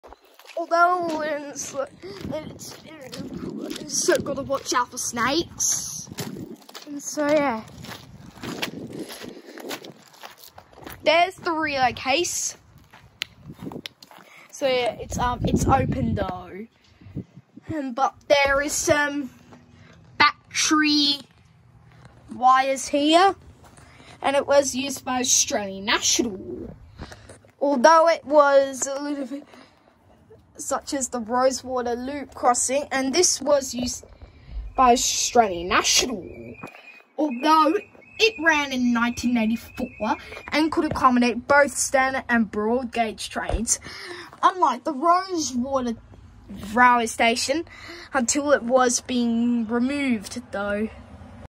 Short rough walk around the Rosewater relay cases
The relay cases are made by these. Rosewater loop railway of things on the paper was made by Australian National.